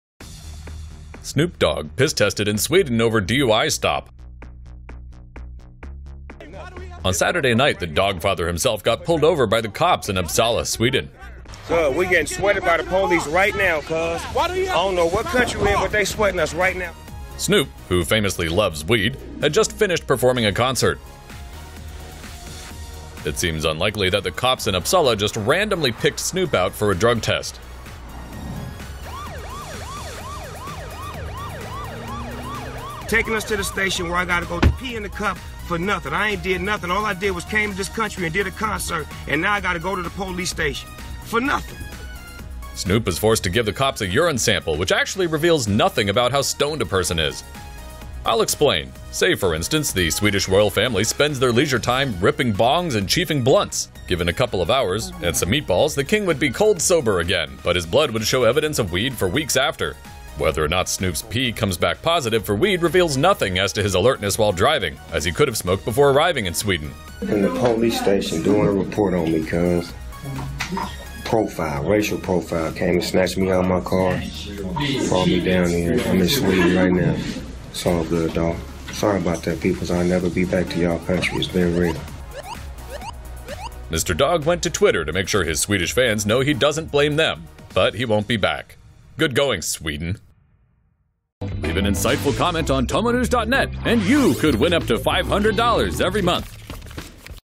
Snoop Dogg arrested in Sweden, forced to pee in cup for weed test by Swedish cops - TomoNews
UPPSALA, SWEDEN — On Saturday night, the Doggfather himself got pulled over by the cops in Uppsala, Sweden.

He Instagrammed most of the encounter, saying "We getting sweated by the police right now, Cuz. I don't know what country we in, but they sweatin us right now."

Snoop, who famously loves weed, had just finished performing a concert. Given his notoriety, it seems unlikely that the cops in Uppsala just randomly picked Snoop out for a drug test.

"...takin' us to the station where I gotta go pee in a cup, for nothin', I ain't did nothin! All I did was I came to this country and did a concert, and now I gotta go to the police station. For nothin!"

Snoop was forced to give the cops a urine sample, which actually reveals nothing about a person's level of intoxication at the time of testing. I'll explain. Say, for instance, the Swedish Royal Family spends their leisure time ripping bongs and chiefing blunts. Given a couple hours and some meatballs, the King would be cold-sober again, but his blood would show evidence of weed for weeks after.

Whether or not Snoop's pee comes back positive for weed reveals nothing as to his alertness while driving, as he could have smoked before arriving in Sweden.
"In the police station doing a report on me, Cuz. Profile, racial profile. Came and snatched me outta my car. Brought me down here, I'm in Sweden right now. It's all good though. Sorry about that, peoples, I'll never be back to y'all country. It's been real."

Mr. Dogg went to Twitter to make sure his Swedish fans know he doesn't blame them, but he won't be back. Good going, Sweden.